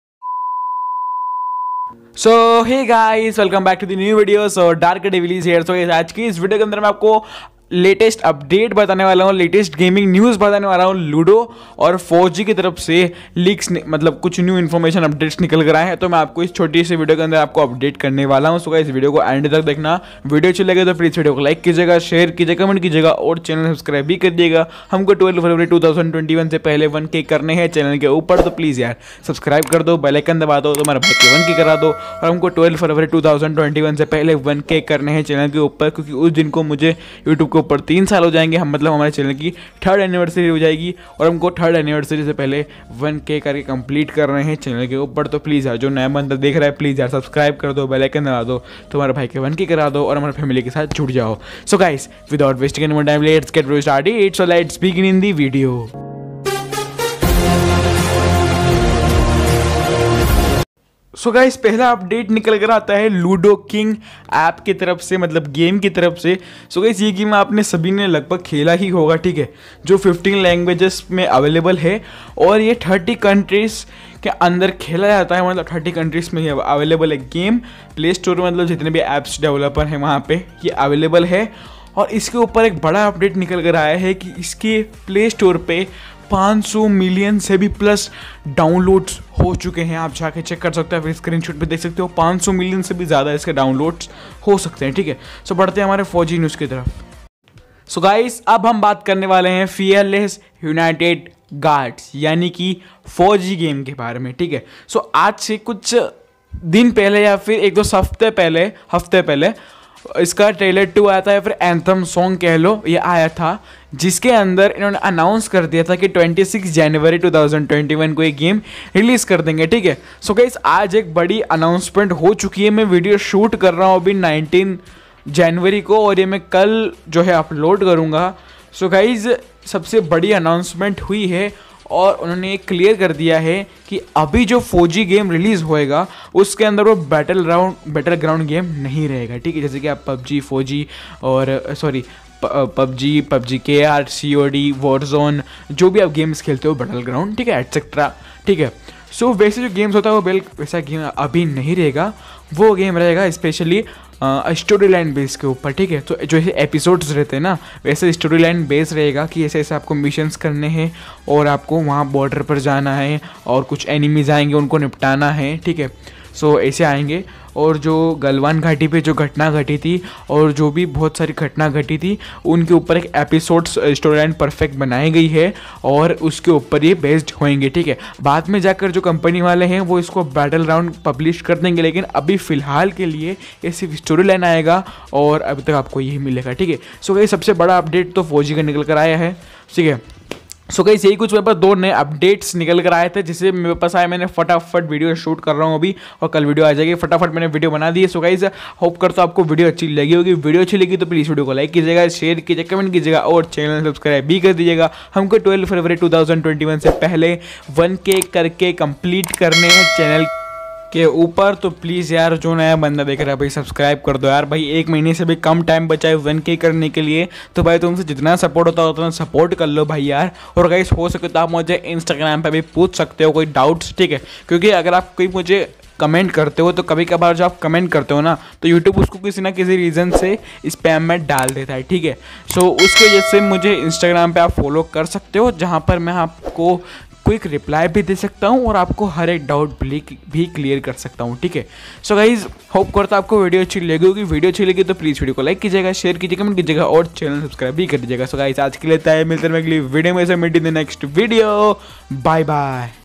GAMING NEWS ABOUT FAUG 🔥🔥
HEY GUYS

SO TODAY I GO TO SHOW YOU THE NEW GAMING NEWS ABOUT FAUG AND LUDO KING

BACKGROUND GAMEPLAY CREDIT-NO COPYRIGHT GAMEPLAY (NCG)